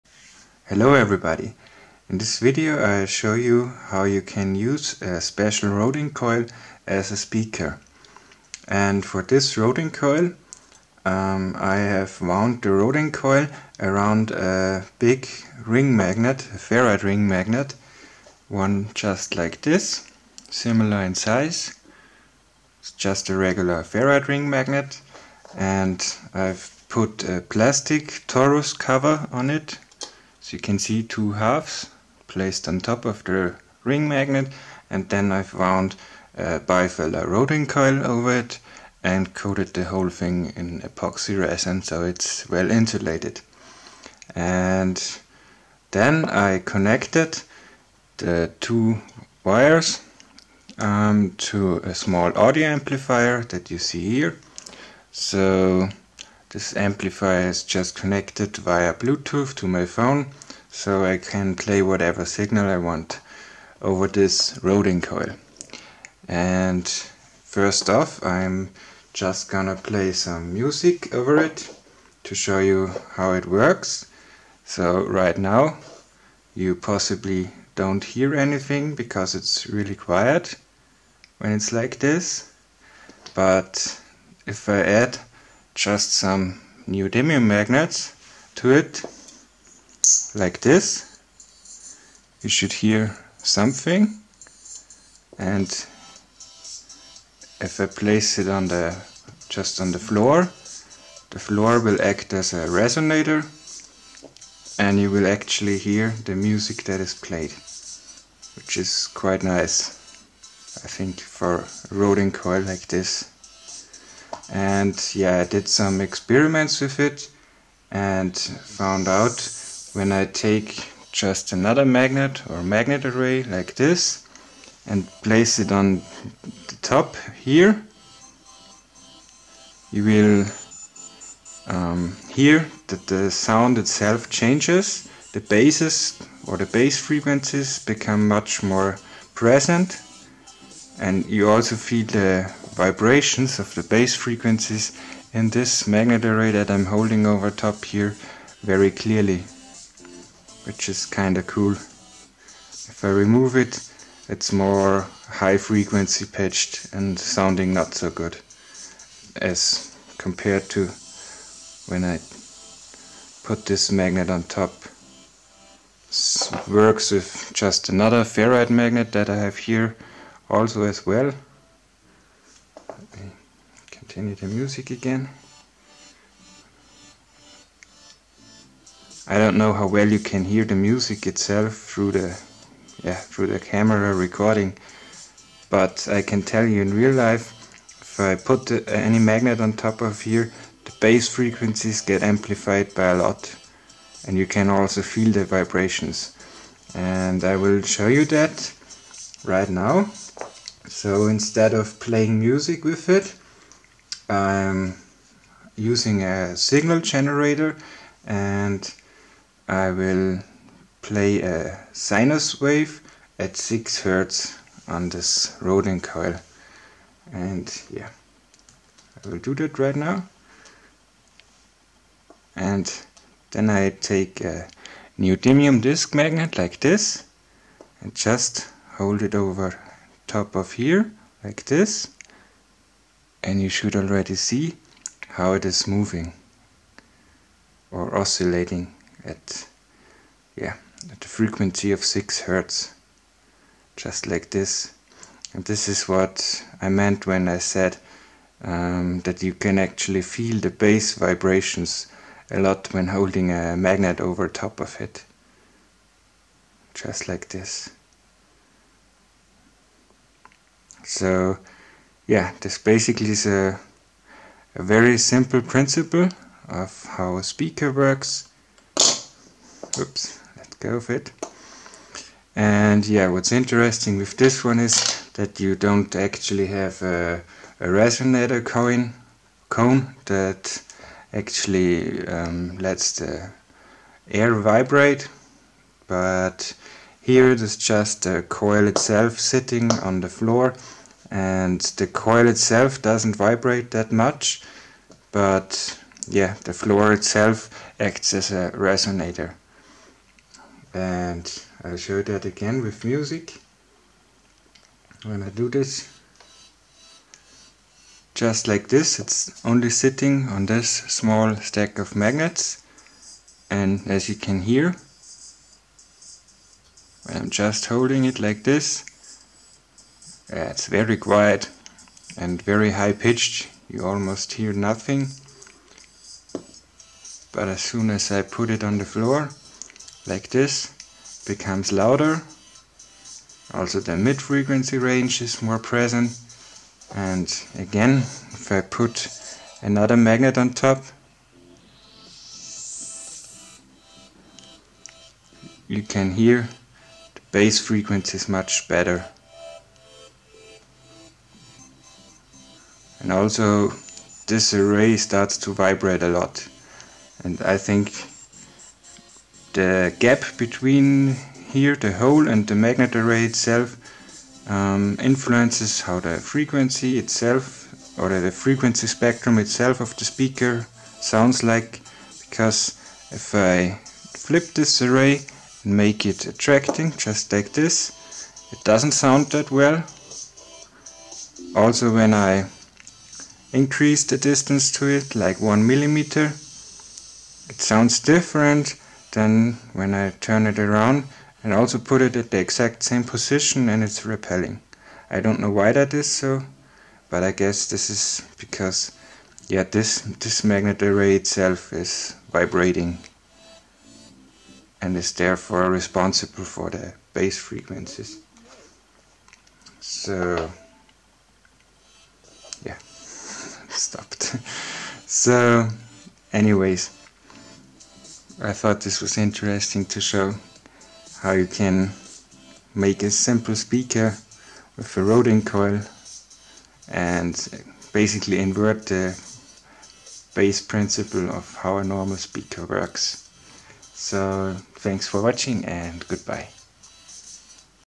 Magnetic Rodin coil used as a speaker - special coil with a ring magnet inside
In this video I show you how you can use a special rodin coil with a ferrite ring magnet inside as a speaker and how you can amplify and manipulate the frequency spectrum of it by adding additional magnets to it.